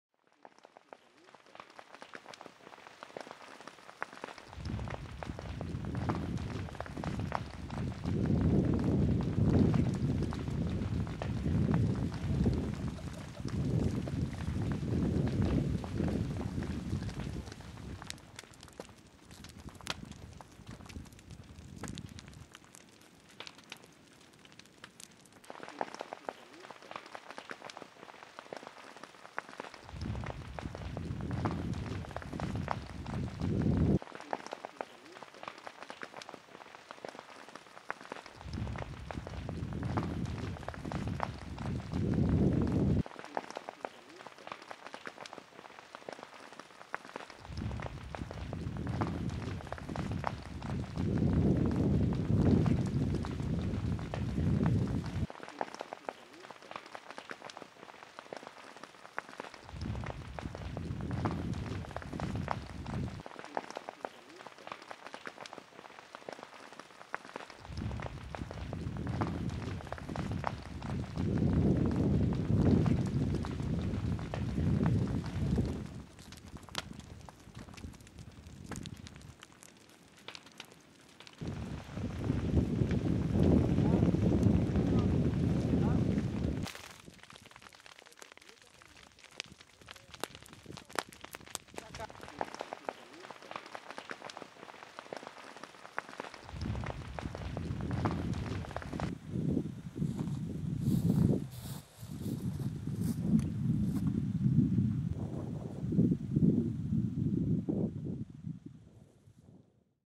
Brésil : la forêt amazonienne brûle toujours
Depuis des semaines, la forêt amazonienne est en proie à de violents incendies imputés, par les autorités brésiliennes, au brûlis, une pratique illégale de défrichement des terres agricoles par le feu.